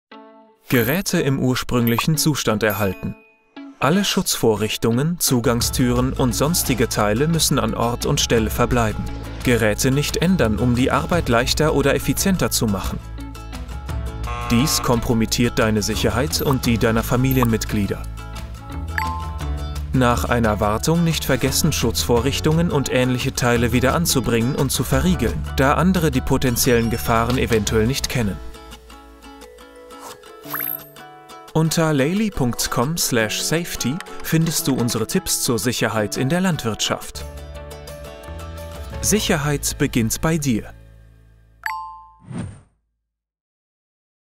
Sicherheit im Stall - Tipp 3: Geräte im ursprünglichen Zustand erhalten
Alle Schutzvorrichtungen, Zugangstüren und sonstige Teile müssen an Ort und Stelle verbleiben. Geräte nicht ändern, um die Arbeit leichter oder effizienter zu machen. Dies kompromittiert Deine Sicherheit und die Deiner Familienmitglieder. Nach einer Wartung nicht vergessen, Schutzvorrichtungen und ähnliche Teile wieder anzubringen und zu verriegeln, da andere die potenziellen Gefahren eventuell nicht kennen.